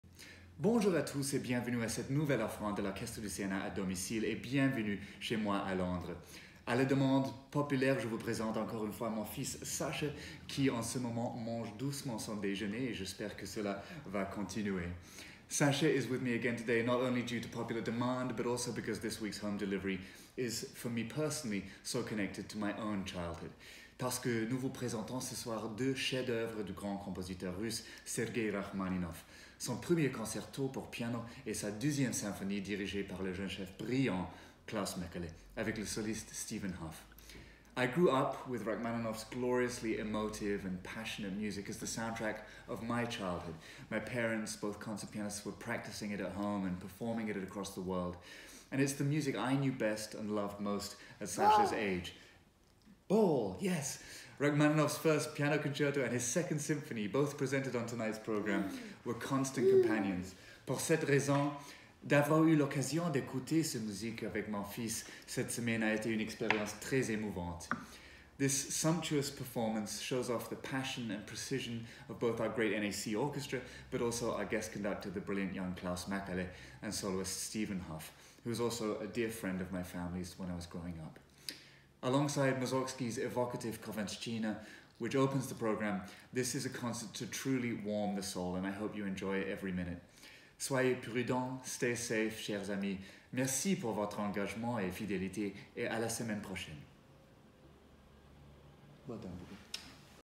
NACO Home Delivery 6: Rachmaninov with Mäkelä and Hough
As the Covid-19 crisis continues, the National Arts Centre Orchestra is each week releasing a never-before-heard recording from their live concert archive.

This week Alexander Shelley introduces the sixth of these concerts, featuring music by Mussorgsky and Rachmaninov with the NAC Orchestra, conductor Klaus Mäkelä and pianist Stephen Hough.